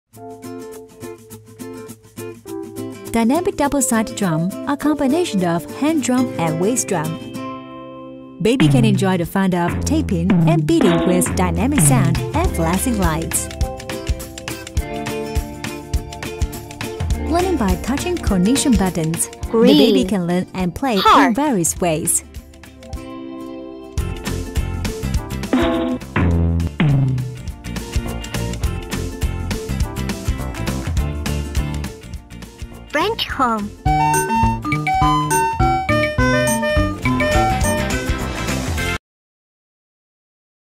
Goodway Double Side Drum
Goodway two-sided drum has 3 ways to play, learn & cognition. Beat drum and play piano, enjoy music and songs. Ideal gift to little one and will surely provide endless hours of fun.